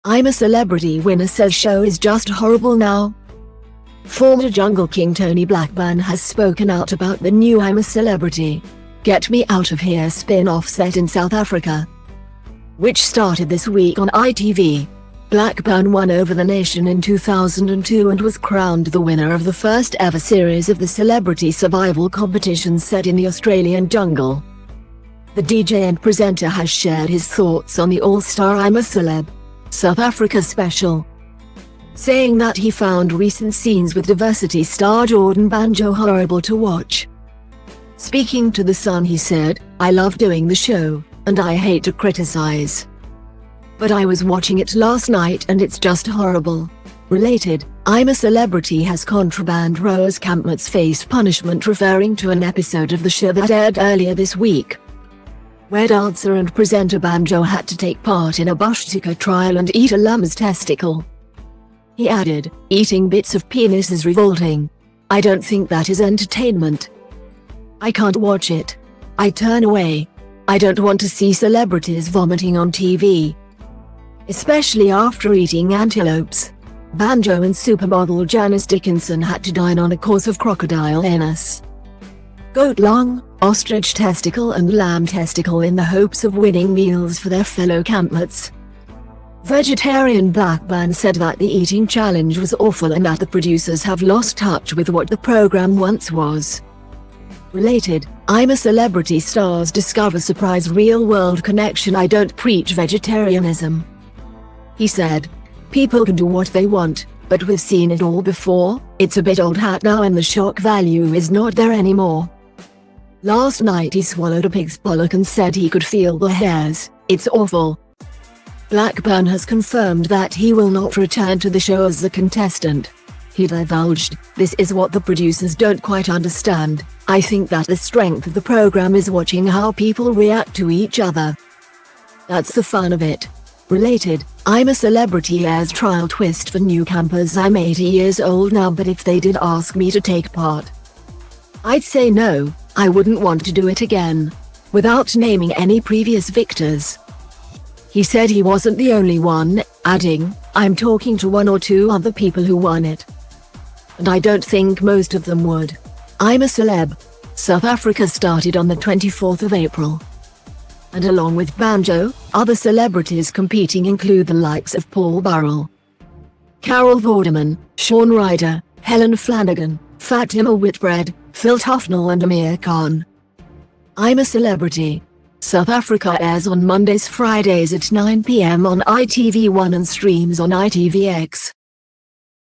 I'm A Celebrity winner says show is 'just horrible' now
Former jungle king Tony Blackburn has spoken out about the new I'm a Celebrity… Get Me Out of Here! spin-off set in South Africa,which started this week on ITV. Blackburn won over the nation in 2002 and was crowned the winner of the first-ever series of the celebrity survival competition set in the Australian jungle.The DJ and presenter has shared his thoughts on the all-star I'm A Celeb. . . South Africa special,